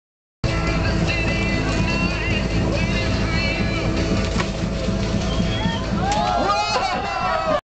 One Way Butter Battle Front Flip
I ended pulling off a front flip at the One Way Butter Battle. It was so scary.